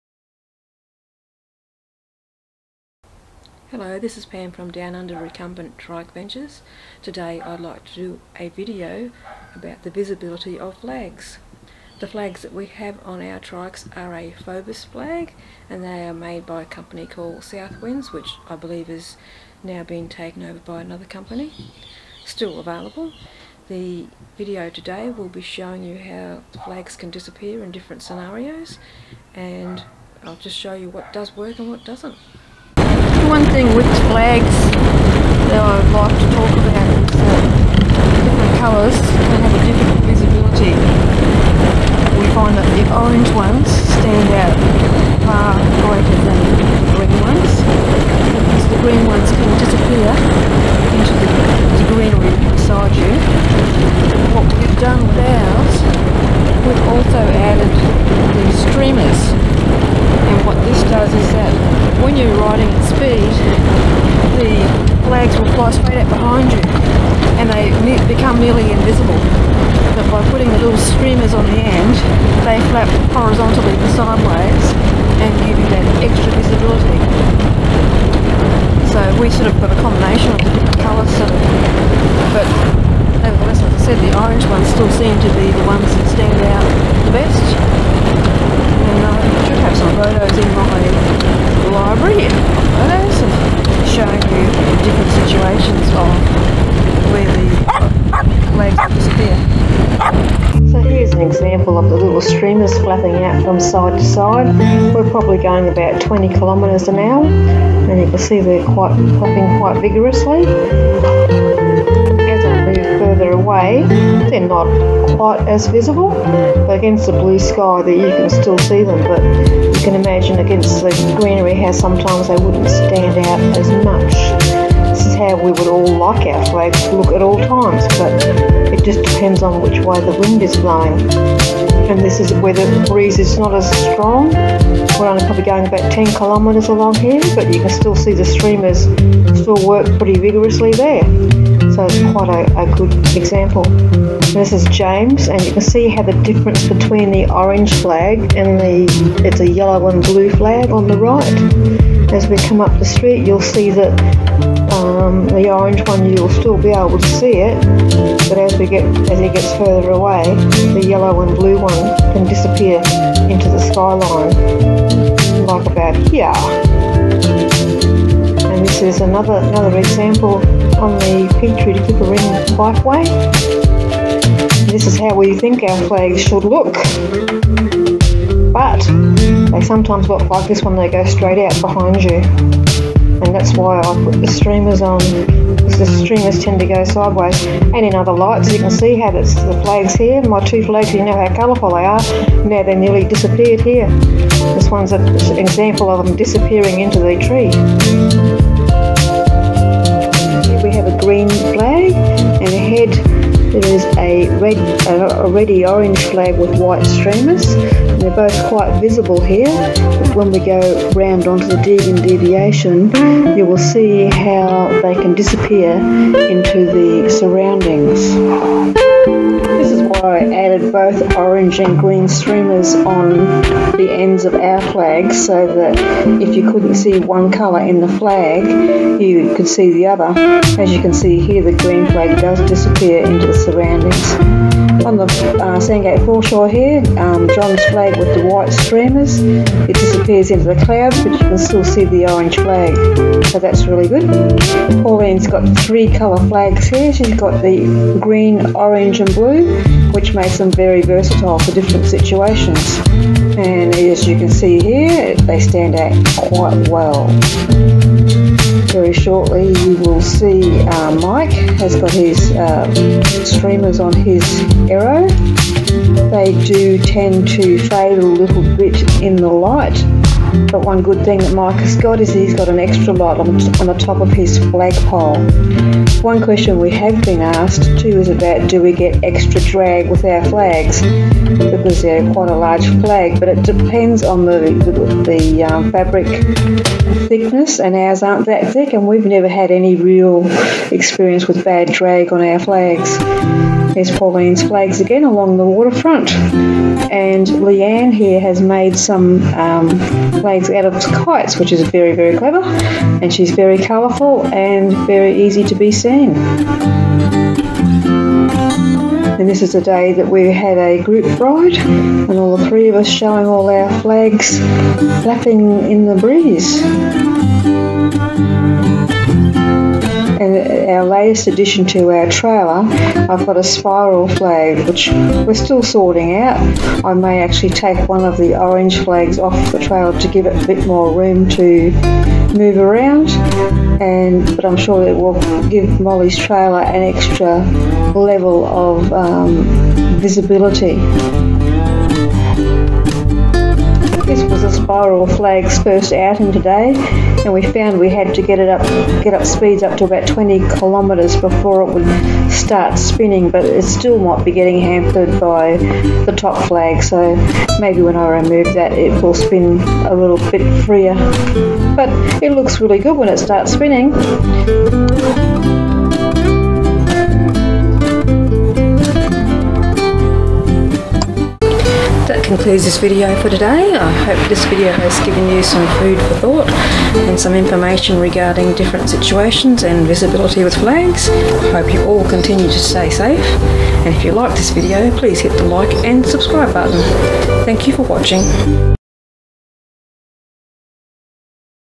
RECUMBENT TRIKE FLAG VISIBILITY
I put this video together to show the visibilities of flags in different situations and how the addition of streamers can create greater visual impact. I think my flags are very colorful, but i can show you in this video how they can disappear.